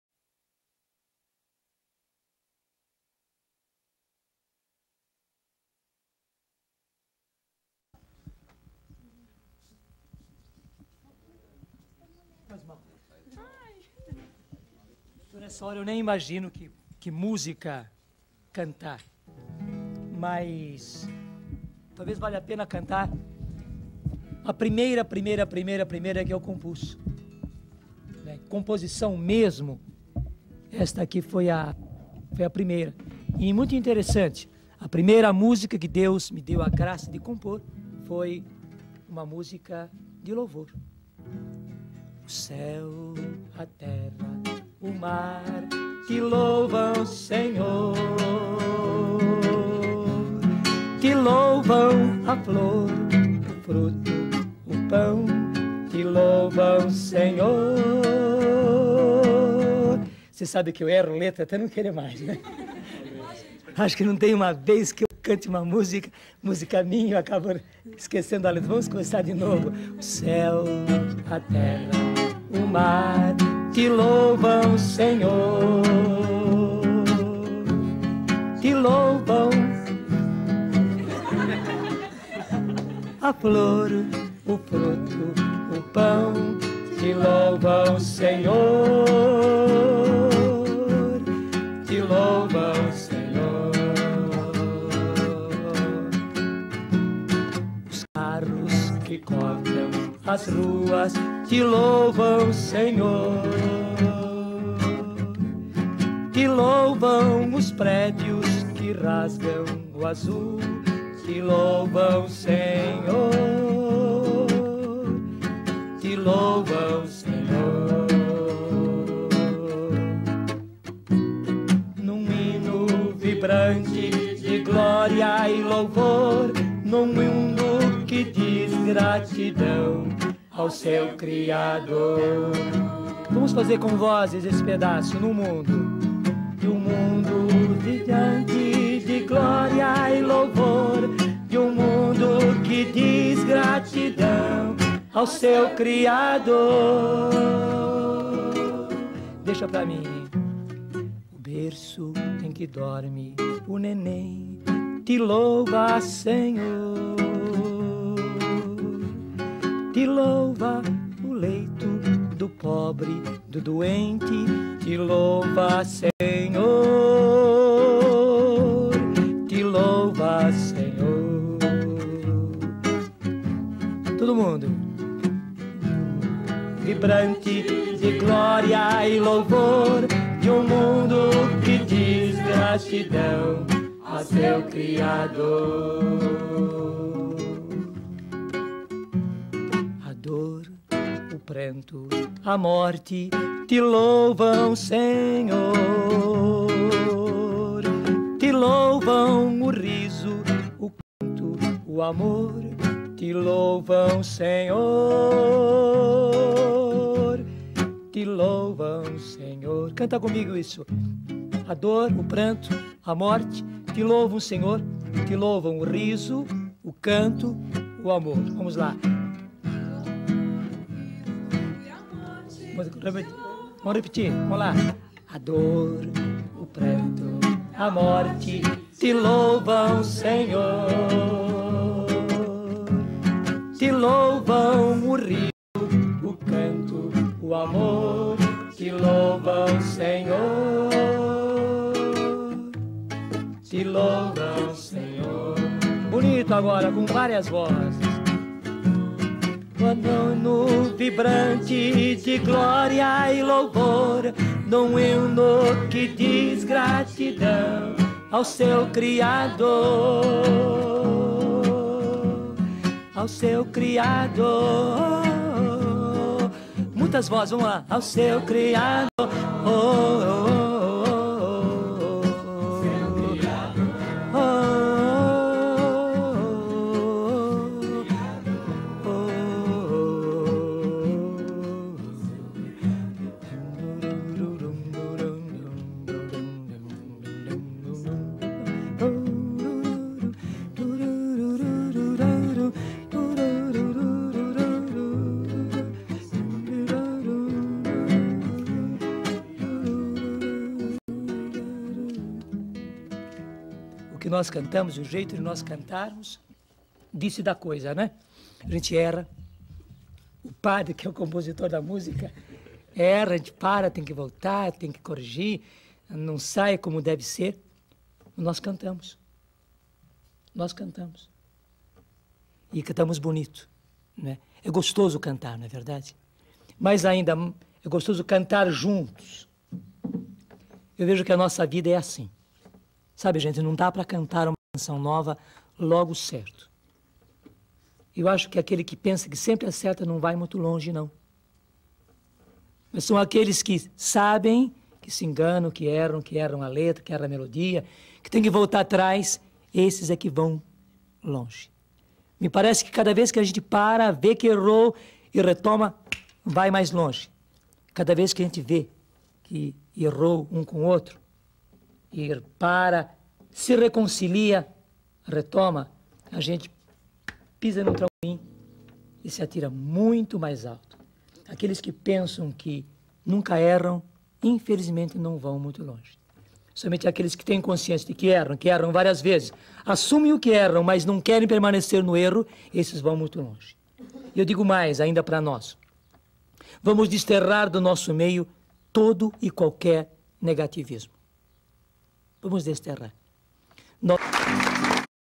Monsenhor Jonas Abib e sua primeira canção
Primeira canção cantada por monsenhor Jonas Abib. Uma música de louvor, que Deus lhe deu a graça de compor.